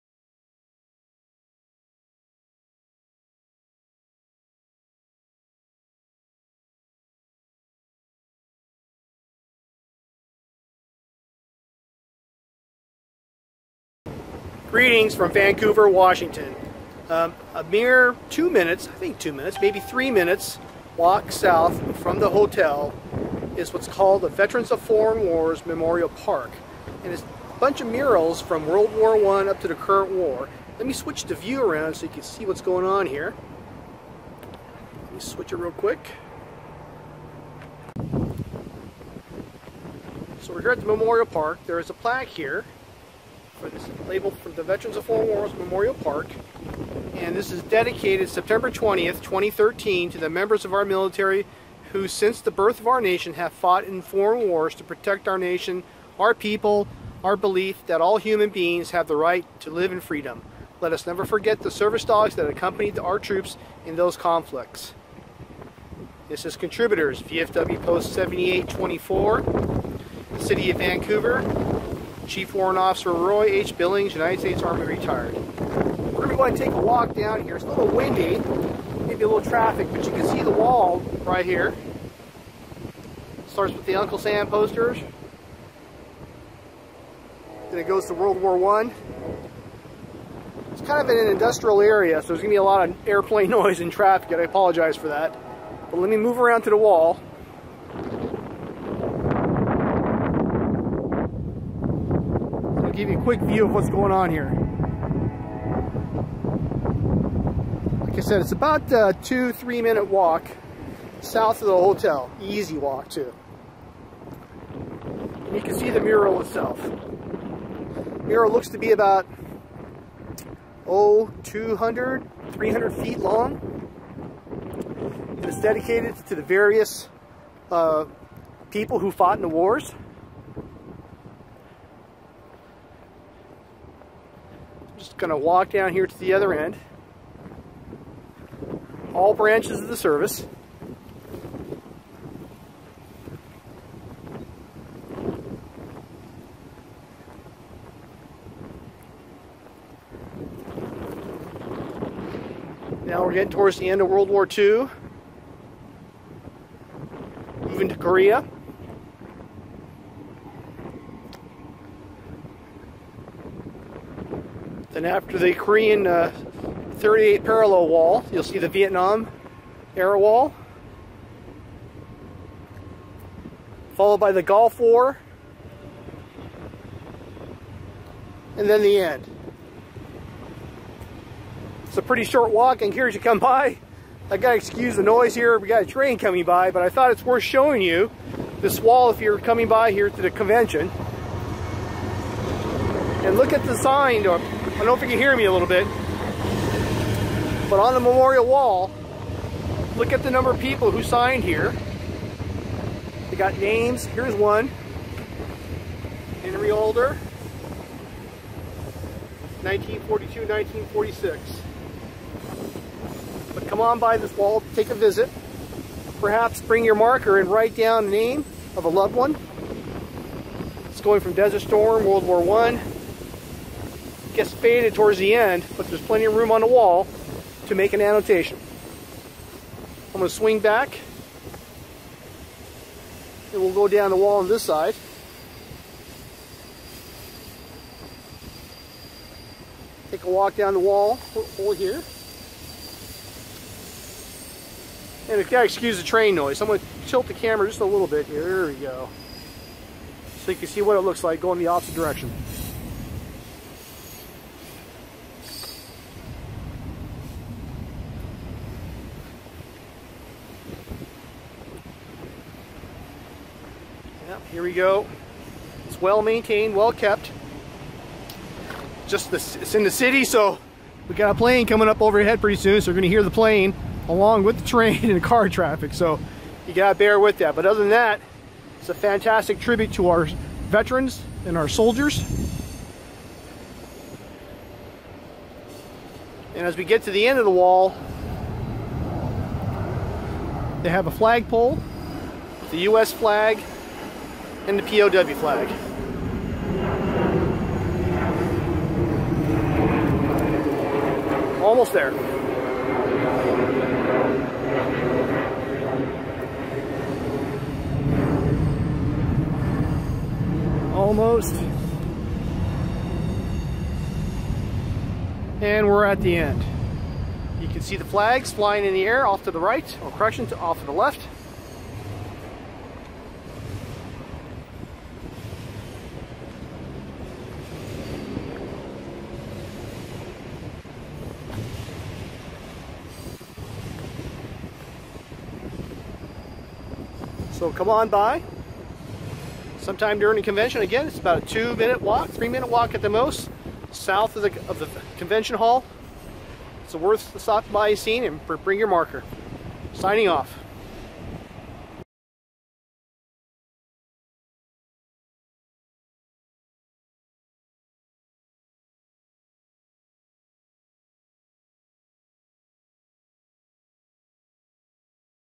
VFW Memorial Wall Vancouver WA
Greetings from Vancouver, WA!  Here is the first video for the VFW Department of Washington June 2019 Convention.  This year the convention is at the Hilton in Vancouver, WA. We are centrally located to many interesting and fun places to visit during the convention, but let me start with the VFW Memorial Plaza.  The plaza is home to the VFW Memorial Wall.  The wall shows murals from WWI to the current war on terrorism. It is a short two to three minute walk south of the Hilton Hotel and it is worth the walk.  Also bring a Sharpies to write a loved one or friends name from America's Wars on the memorial wall.  Note one of the contributors to the plaza is VFW Post 7824 in Vancouver, WA.